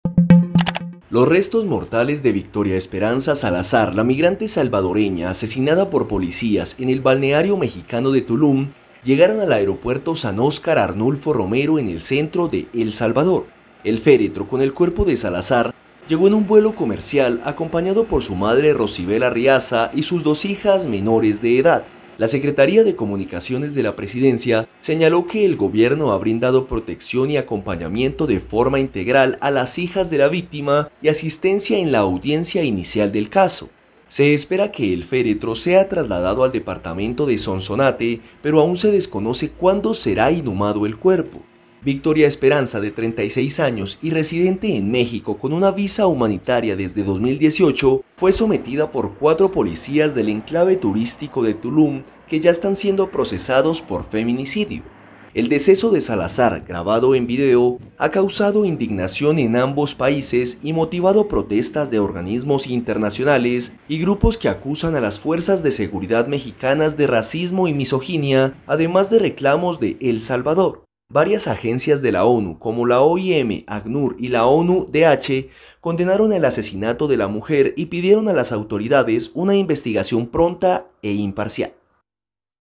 Arriban a El Salvador los restos de Victoria Salazar, asesinada por policías en México
Los restos mortales de Victoria Esperanza Salazar, la migrante salvadoreña asesinada por policías en el balneario mexicano de Tulum, llegaron este domingo al aeropuerto San Óscar Arnulfo Romero, en el centro de El Salvador, confirmó a Efe un portavoz gubernamental.

¡Visítenos!